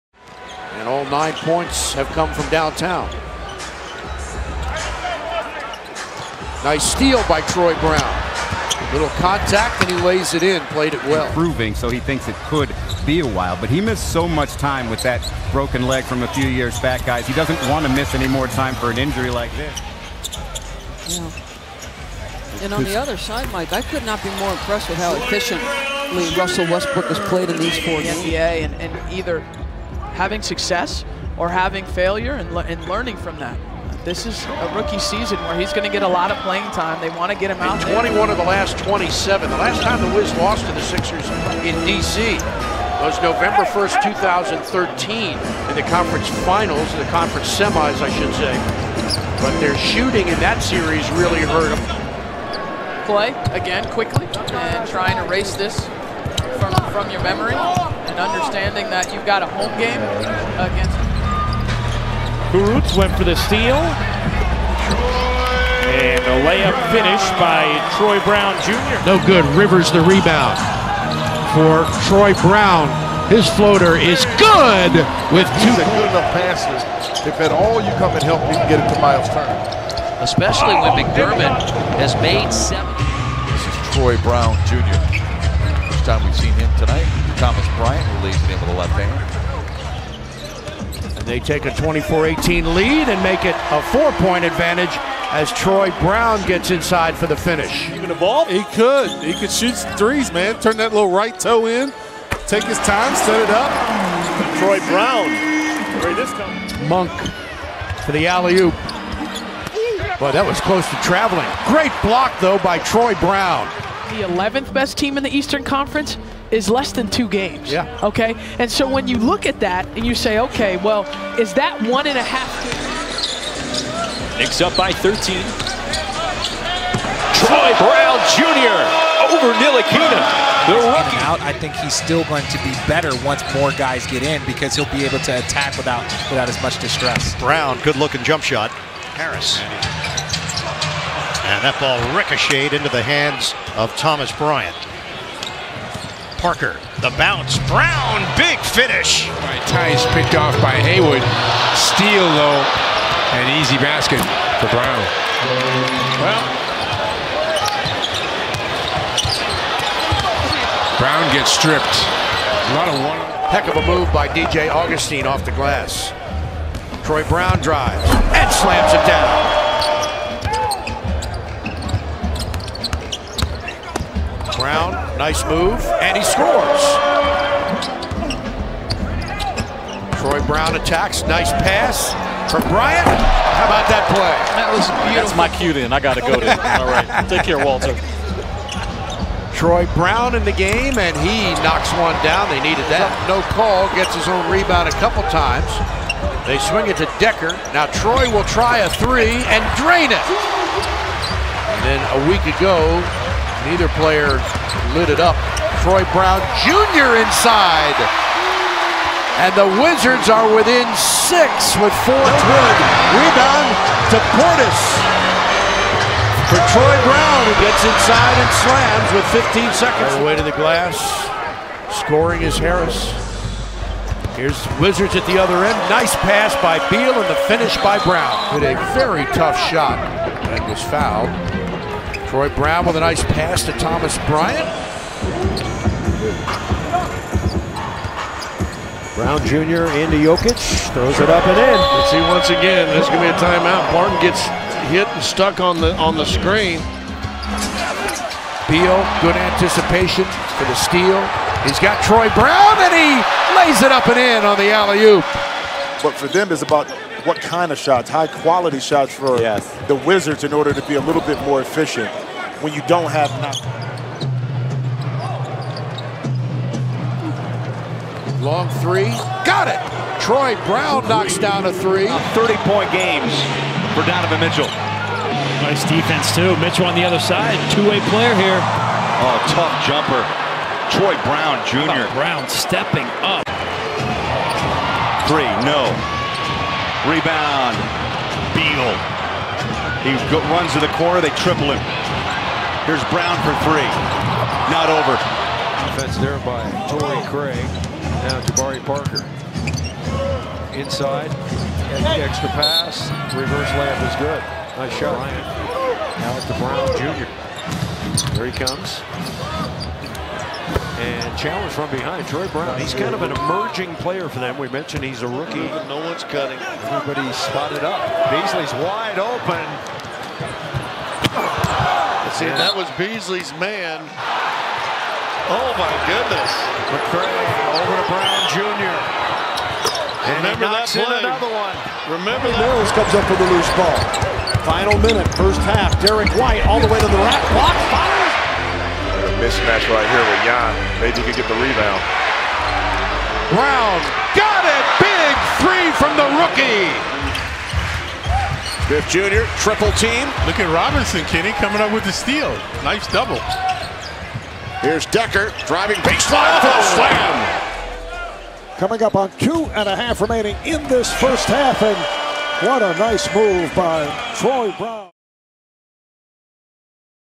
Troy Brown Jr. 2018-19 Season Highlights | 15th Pick '18 [HD]
After getting liimited minutes early in the season, Troy Brown Jr. flashed his potential late in the season.

Hoop Intellect Highlights

Highlighting Notable Performances

Follow @HoopIntelllect on Twitter